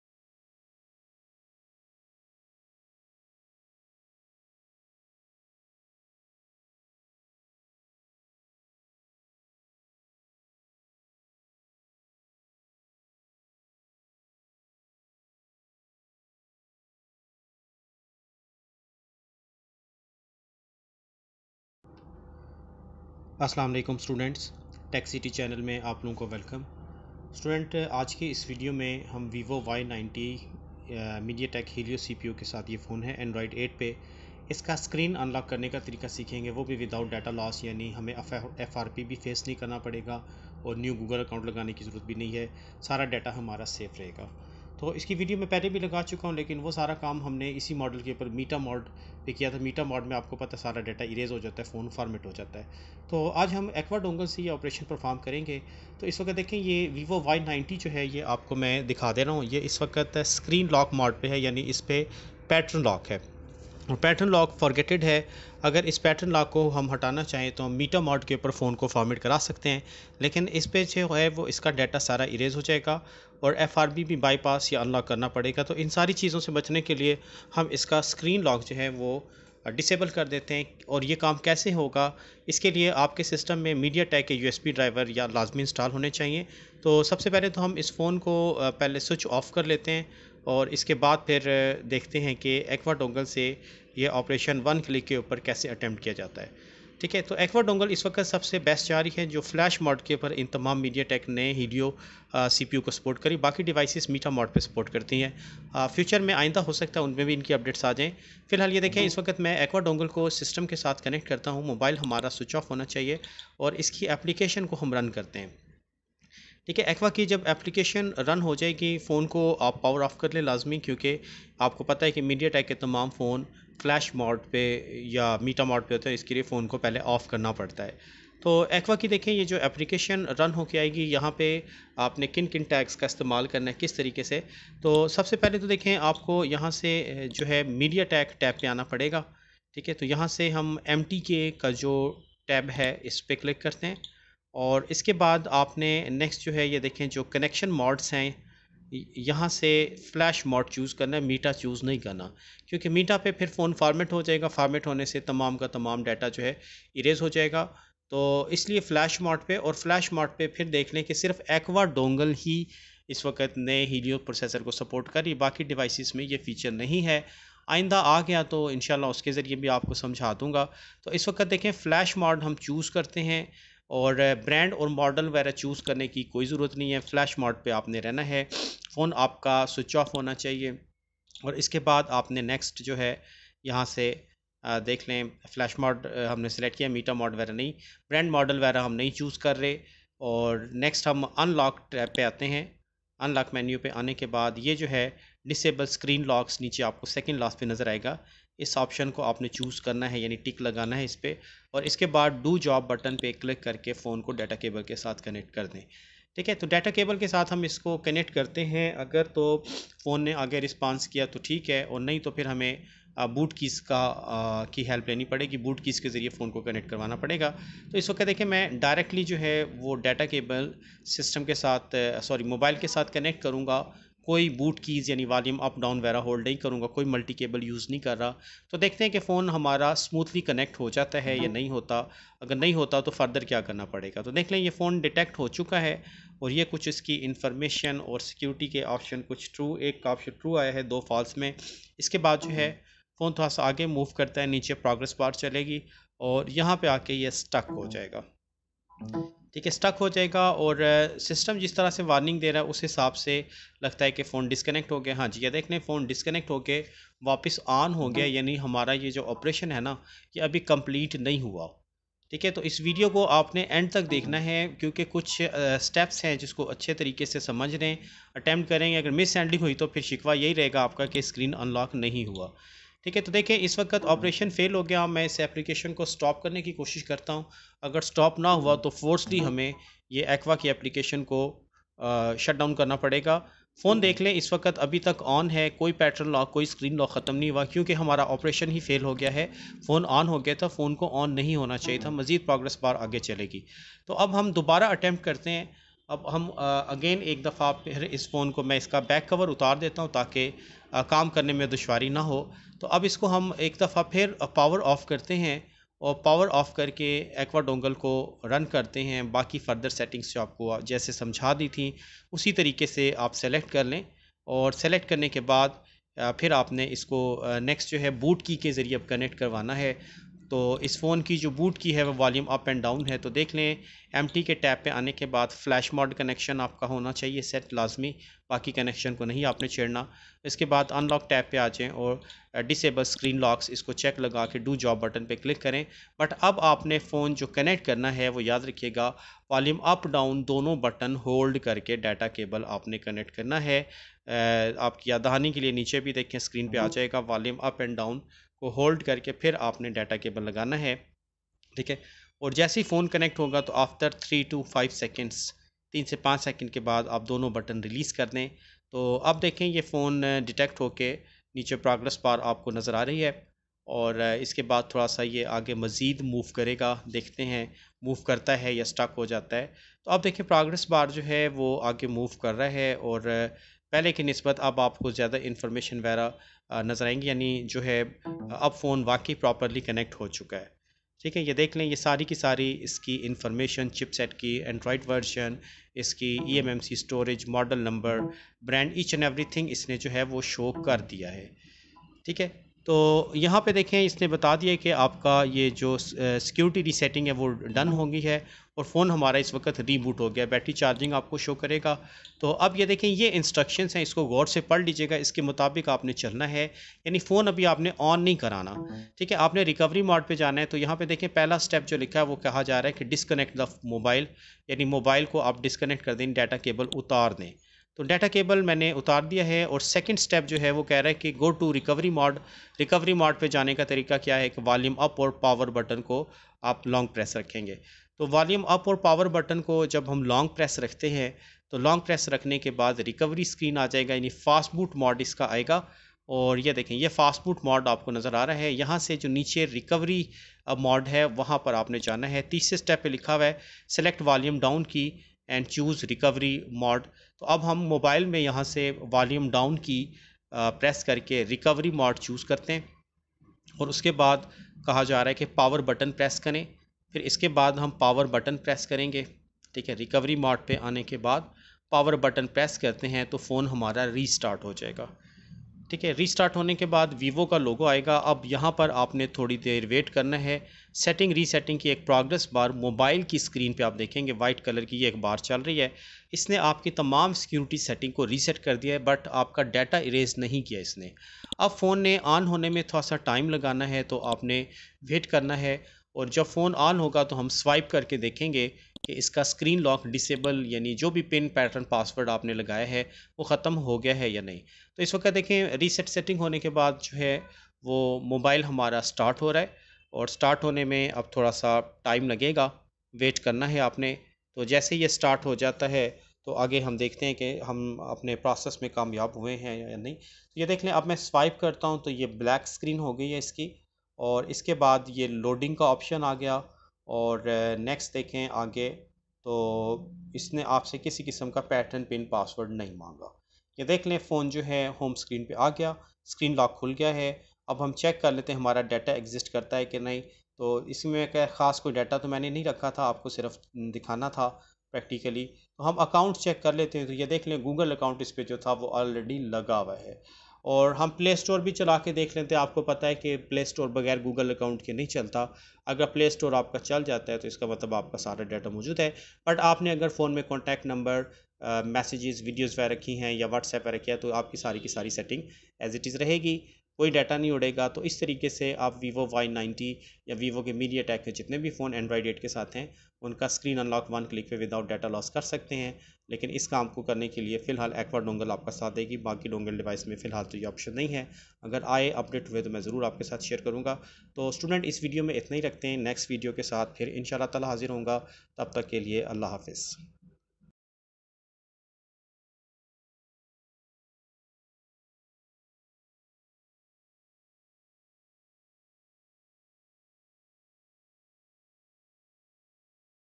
How to remove Vivo Y90 screen lock w/o data loss android 8.x | Hindi/Urdu | TECH City
This video we learn that how remove vivo y90 pin/patter/password without data loss via Aqua Dongle android 8.x with mediatek helio cpu